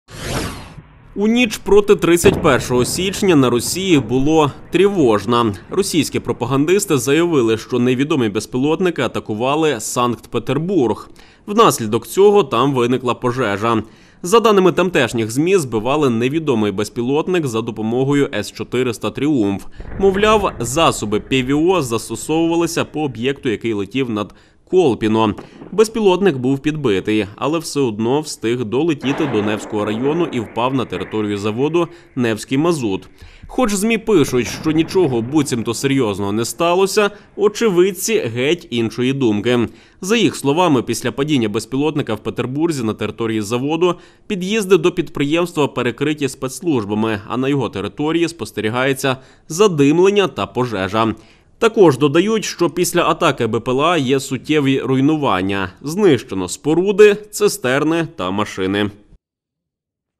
Трєвожна ніч на росії😈😈дрон АТАКУВАВ Санкт-Петербург, сталась пожежа на заводі
Ніч проти 31 січня на росії була – трєвожна. Російські пропагандисти заявили, що невідомі безпілотники атакували Санкт-петербург.